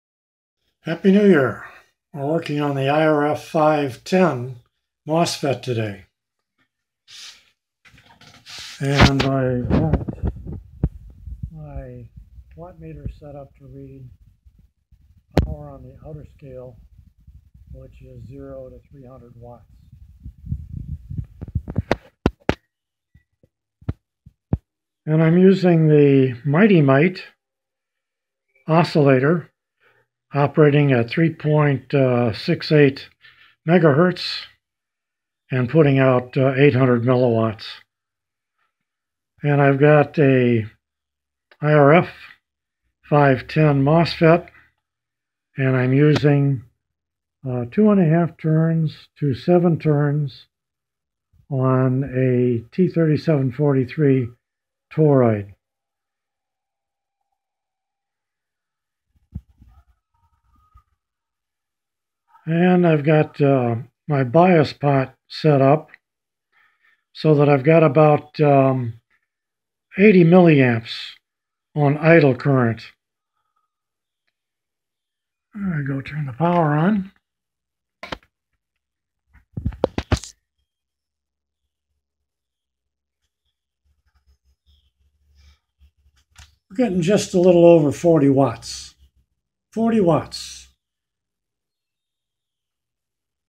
IRF510 MOSFET HF POWER AMPLIFIER 40 WATTS
First prototype of IRF510 MOSFET device operating with 800 mw drive using Michigan Mighty Mite Power Oscillator. Developing 40 Watts of power output into a 50 Ohm load.